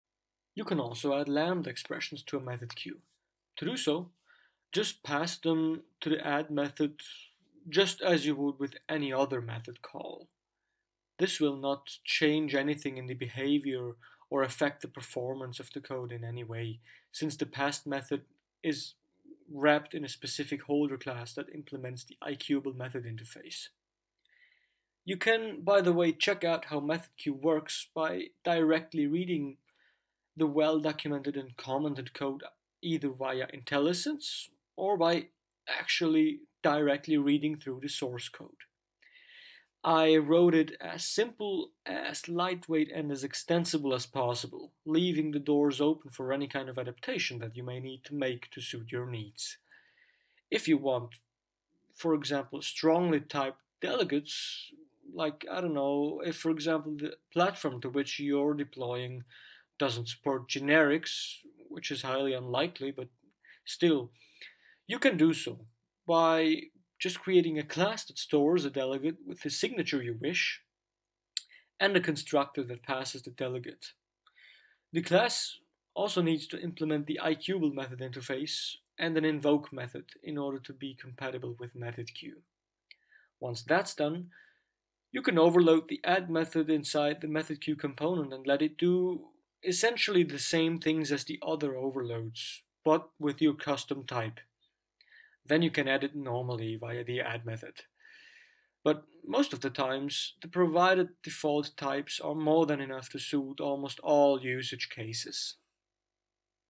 MethodQ: Lambda expressions and extensibility
You can also add lambda expressions to your method queues. This can save you both time and effort when writing your code (not having to write out the methods to a class).
They can make your code so much more readable.
Furthermore, it is possible to extend MethodQ easily by using the provided interface, such that you can add custom delegate types to queues (or even modify MethodQ directly to have it suit your own personal needs for your projects).